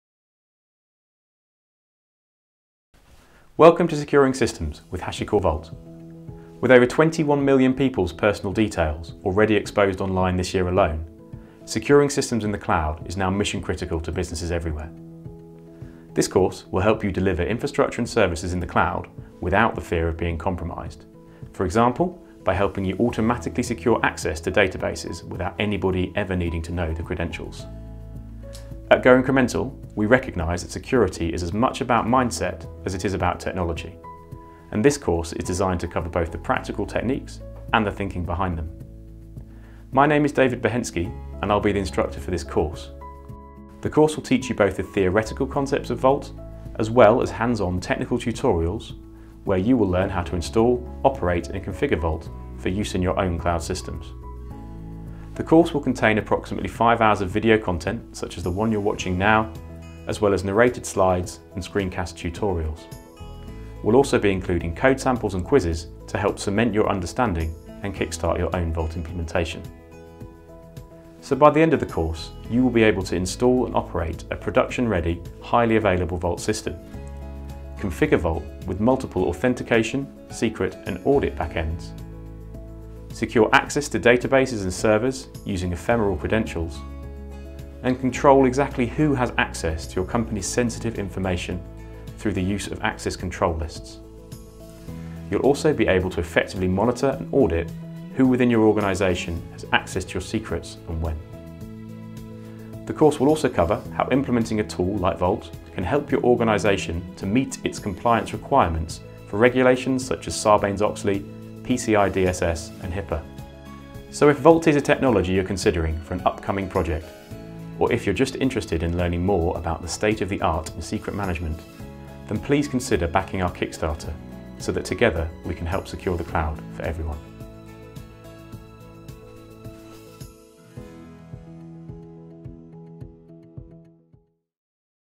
Securing system with Hashicorp Vault - Kickstarter Video
Go Incrementals Kickstarter video in order to raise funds to create a quality course to teach users about Vault